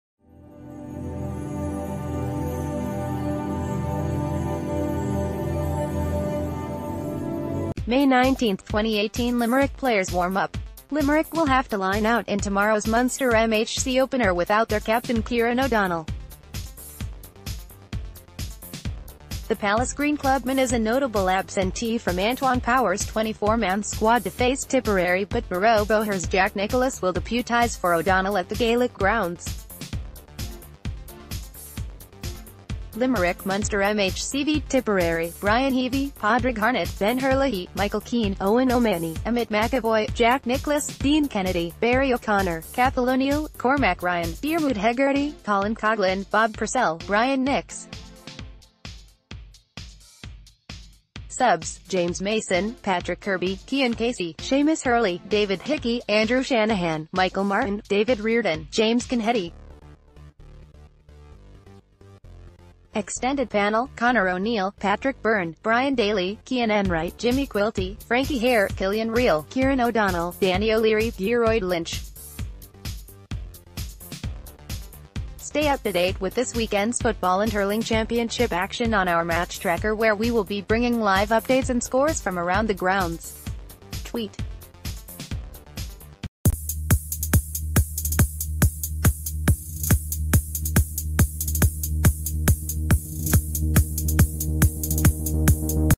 Breaking News | Team news: Limerick minors without captain
May 19, 2018 Limerick players warm up. Limerick will have to line out in tomorrow's Munster MHC opener without their captain Kyran O'Donnell. The Pallasgreen clubman is a notable absentee from Antoin Power's 24-man squad to face Tipperary but Murroe-Boher's Jack Nicholas will deputise for O'Donnell at the Gaelic Grounds. Limerick (Munster MHC v Tipperary): Bryan Heavey; Padraig Harnett, Ben Herlihy, Michael Keane; Eoin O'Mahony, Emmet McEvoy, Jack Nicholas; Dean Kennedy, Barry O'Connor; Cathal O...